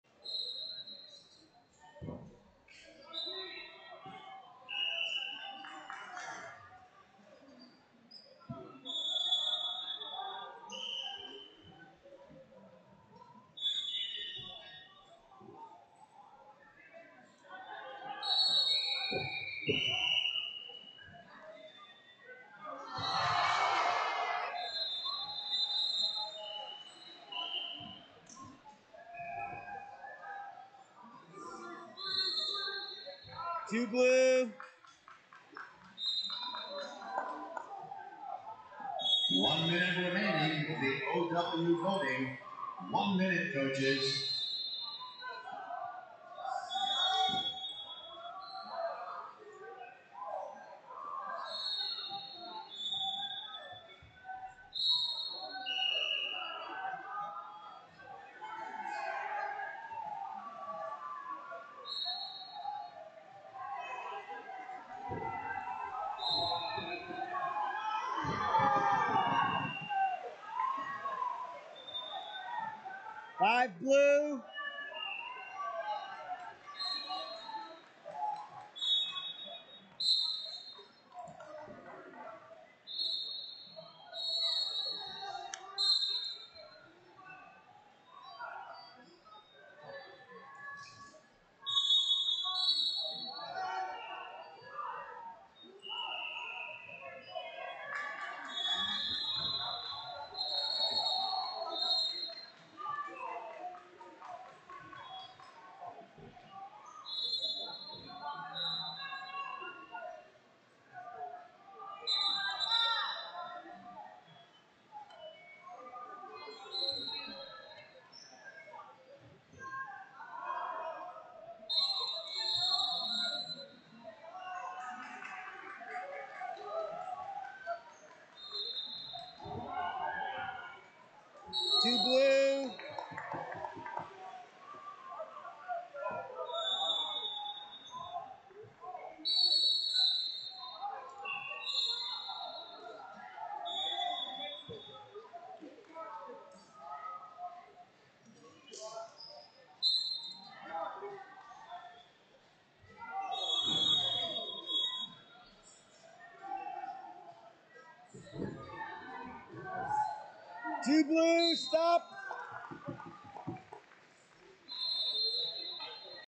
[boutNo] 145 Cole Becker Minnesota Blue Vs Jordan Williams Oklahoma Outlaws FS Red
2021 Junior Duals Freestyle - [boutNo] 145 Cole Becker Minnesota Blue Vs Jordan Williams Oklahoma Outlaws FS Red